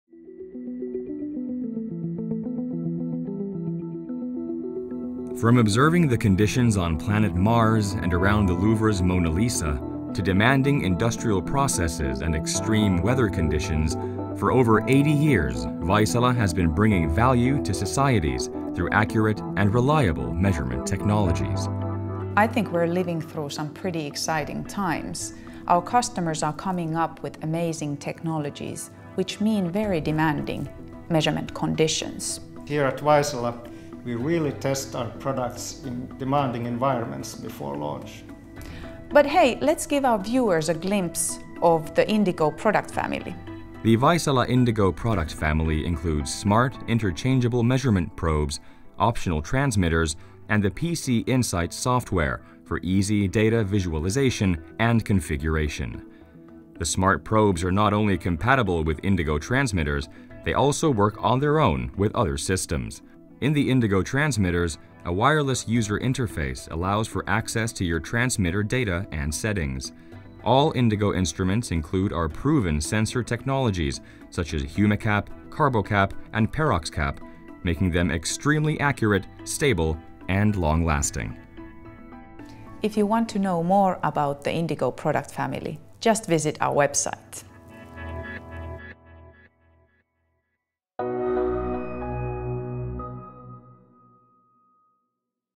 Vaisala Indigo Product Family - Short Introduction
Product Area Manager Maria Uusimaa and Senior Product Manager Lars Stormbom give an introduction to Vaisala Indigo product family for measuring industrial manufacturing processes, life science applications and other demanding measurements. The Indigo family includes smart, interchangeable measurement probes for measuring humidity, carbon dioxide and hydrogen peroxide. It includes optional host devices and Insight PC Software for easy configurations and visualizations.